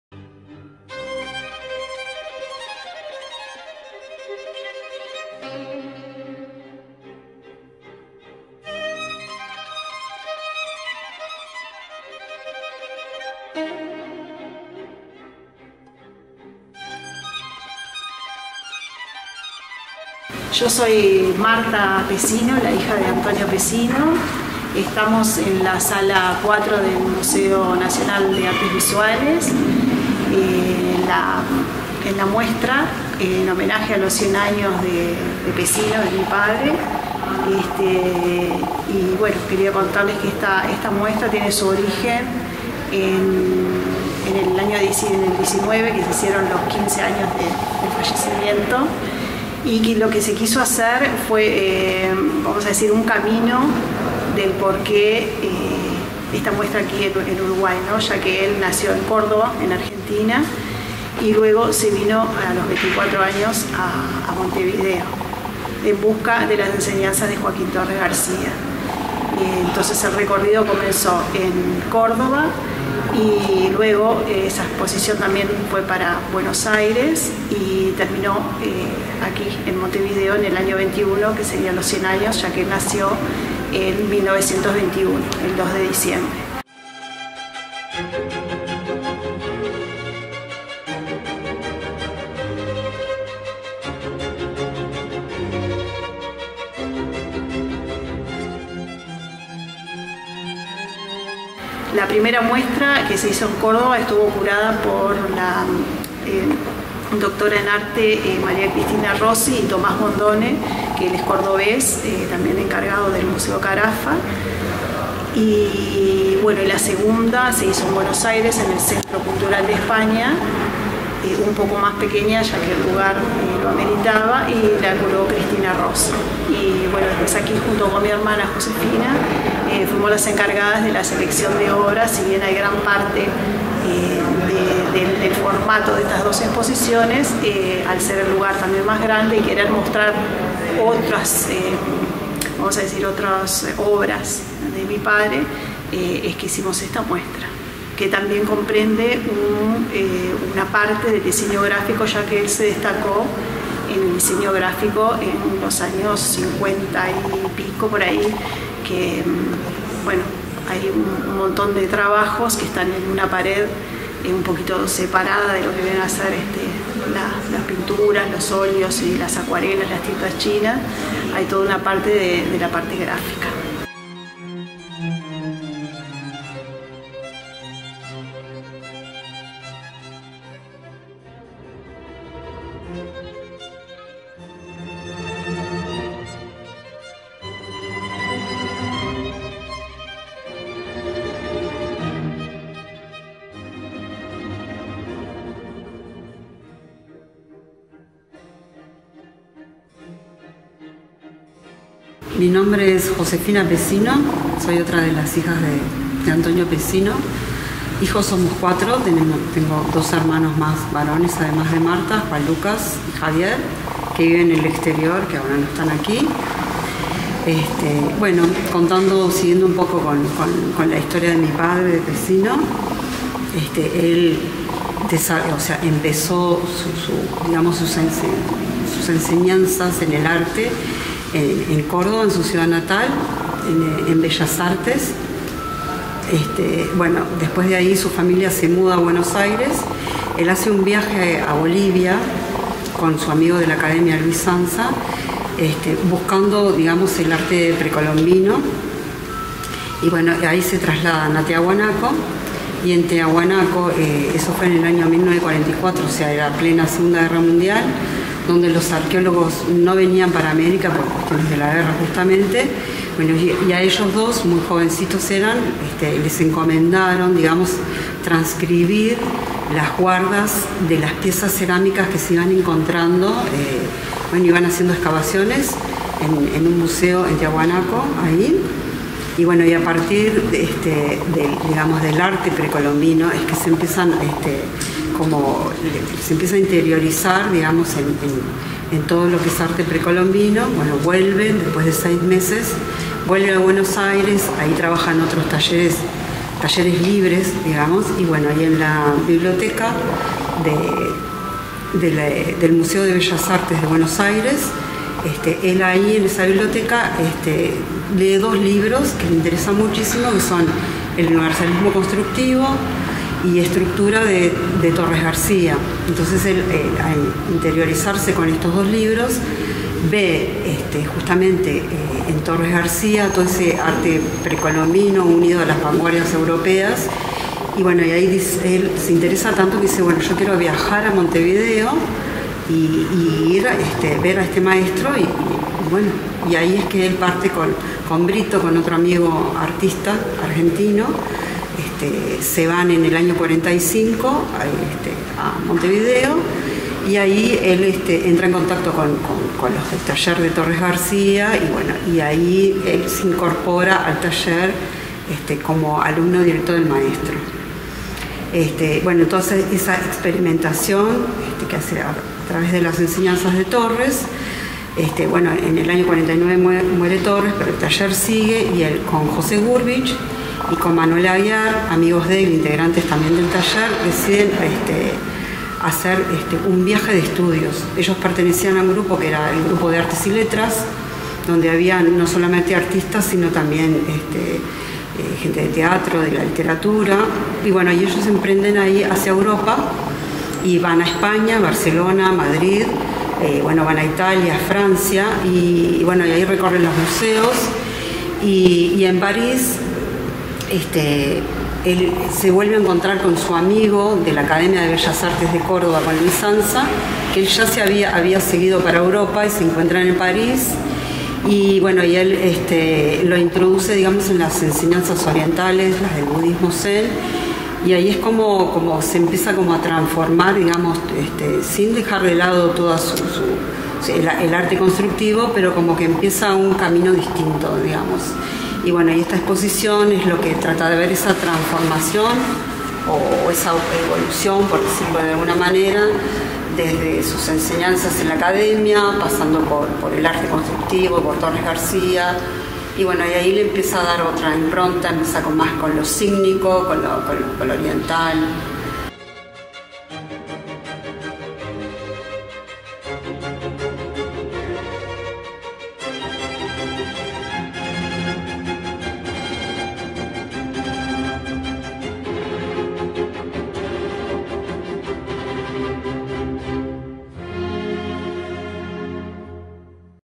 1 Antonio Pezzino en el MNAV. Conversamos con sus hijas Martha y Josefina.
Recorrimos la muestra "Antonio Pezzino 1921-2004 / Retrospectiva en su centenario" en el Museo Nacional de Artes Visuales de Montevideo con Martha y Josefina. Hijas de Antonio Pezzino y curadoras de la muestra.
Ambas siguieron carreras artísticas en disciplinas como cerámica, fotografía y arte textil.
Esta muestra itineró por Cordoba, Buenos Aires y ahora en Montevideo y estará hasta el 2 de mayo.

Pezzino nació en Córdoba en 1921 donde se formó.
En 1945 viajó a Montevideo para incorporarse al taller de Joaquín Torres García.
En 1954 viajó a Francia, Italia y España, donde estudió a los maestros del arte occidental que admiraba y al mismo tiempo, tomó contacto con el arte y la filosofía oriental, ideas que abrieron una nueva dirección en su concepción plástica.

Como artista gráfico, trabajó en la realización de tapas de discos, libros y afiches.
Diagramó la revista Removedor del Taller Torres García y el diario El País, ilustró la sección literaria de este diario, del semanario Marcha y de la revista Tres, entre muchos otros trabajos de diseño gráfico.

En 1980 obtuvo la carta de ciudadanía uruguaya.
Falleció en Montevideo el 29 de abril de 2004 a la edad de 82 años.
La exposición Antonio Pezzino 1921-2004 / Retrospectiva en su centenario va a tener en este canal varios capitulos.